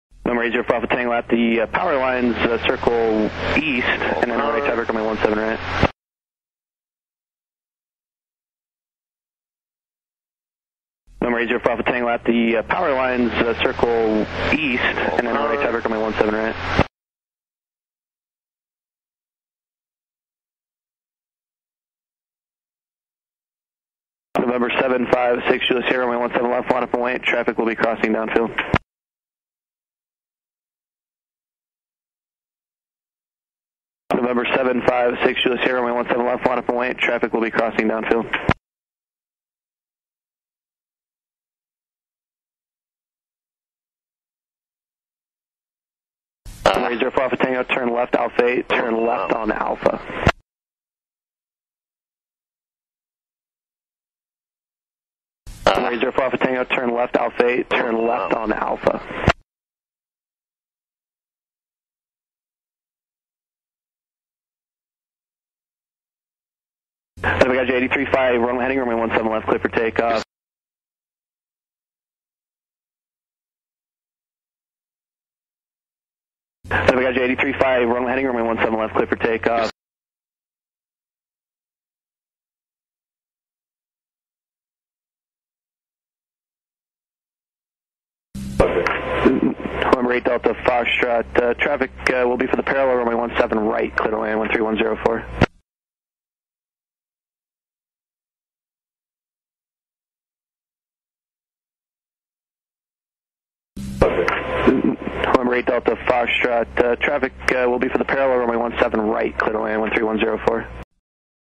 Entrainement gratuit a l'examen FCL055 de compétence linguistique - Ecoute de bande VFR 48
Cet exercice provient du site Lingaero, qui fournit un large éventail d'exercices gratuits pour se former à l'anglais aéronautique, en particulier à l'examen FCL 055. Vous pouvez voir l'exercice complet à